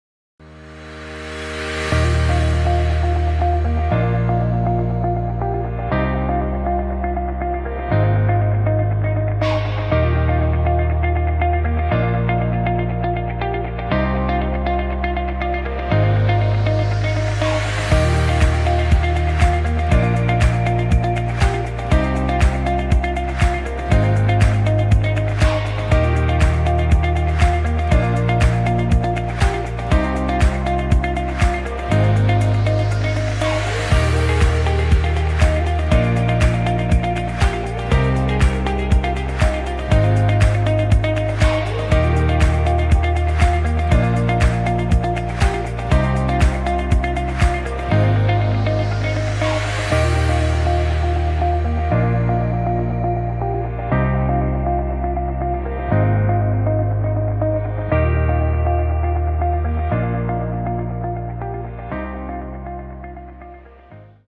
025 Drone Tacacorte, ANEMIS, Garcia,
A small drone movie from the nice and protected marina Tazacorte. ANEMIS and the crew enjoyed the time there.